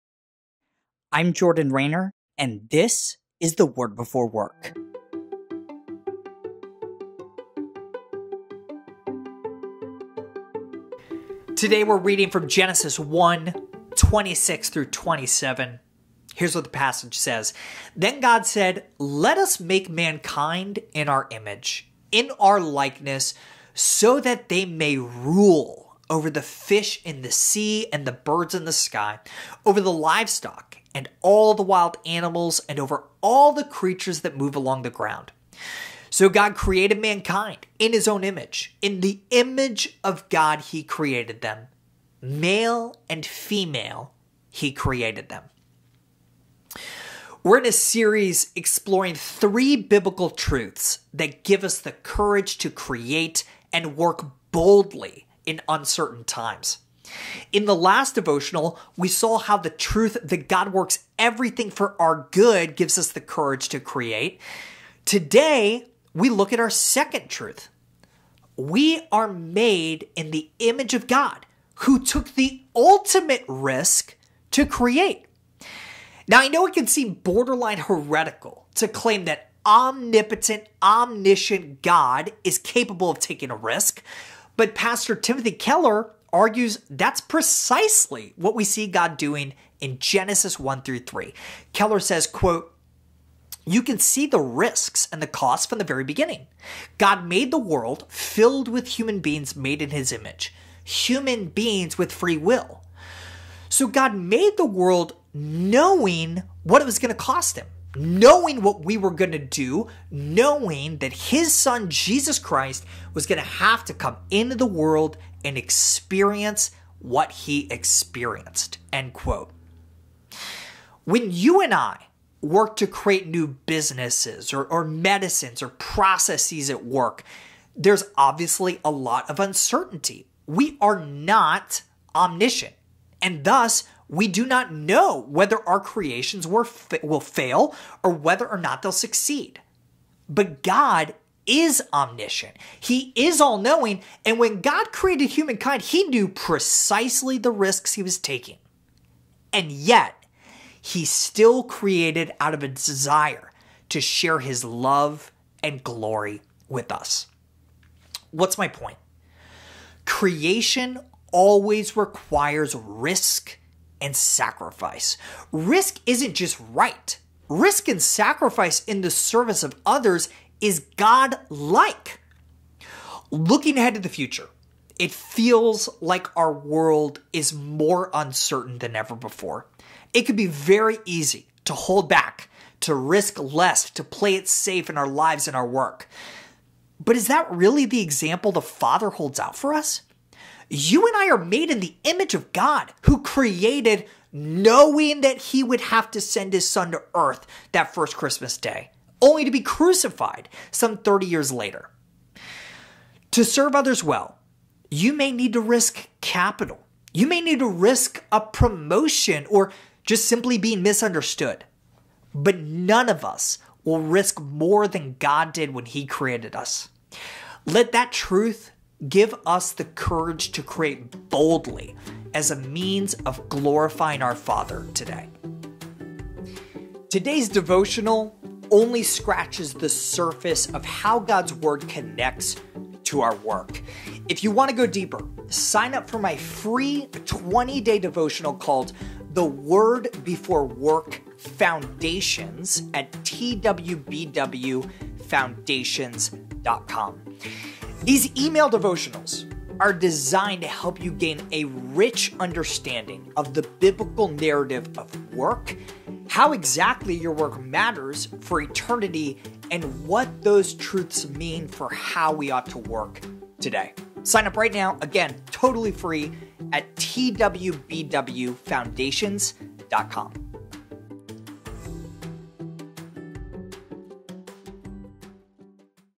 A God Who Risks
Sign-up for my free 20 day devotional, The Word Before Work Foundations,

Series: The Courage to Create
Devotional: 2 of 3

Then God said, “Let us make mankind in our image, in our likeness, so that they may rule over the fish in the sea and the birds in the sky, over the livestock and all the wild animals, and over all the creatures that move along the ground.” So God created mankind in his own image, in the image of God he created them; male and female he created them. (Genesis 1:26-27)

We’re in a series exploring three biblical truths that give us the courage to create and work boldly in uncertain times. Last week, we saw how the truth that God works everything for our good gives us the courage to create. This morning we look at our second truth: We are made in the image of God who took the ultimate risk to create.

It can seem borderline heretical to claim that omnipotent, omniscient God is capable of taking a risk, but pastor Tim Keller argues that’s precisely what we see in Genesis 1-3. Here’s Keller: “You can see the risks and the costs from the very beginning. God made the world filled with human beings made in his image, human beings with free will. So God made the world knowing what it was going to cost him. Knowing what we were going to do. Knowing that [his] Son was going to have to come into the world and experience what he experienced.”

When you and I work to create new businesses, medicines, or processes at work, there is a tremendous amount of uncertainty. We are not omniscient and thus do not know whether our creations will fail or succeed. But God is omniscient. He is all-knowing. When God created humankind, He knew precisely the risks He was taking and yet still created out of a desire to share His love and glory with us.

What’s my point? Creation always requires risk and sacrifice. Risk isn’t just right. Risk and sacrifice in the service of others is God-like.

As we enter 2021, our world is more uncertain than ever. It could be easy to hold back, risk less, and play it safe at work in the New Year. But is that really the example the Father holds out for us? You and I are made in the image of God who created knowing that He would have to send His Son to earth that first Christmas day, only to be crucified some thirty years later.

To serve others well in 2021, you may need to risk capital, a potential promotion, or being misunderstood. But none of us will risk more than God did when He created us. Let that truth give us the courage to create boldly as a means of glorifying our Father in the New Year!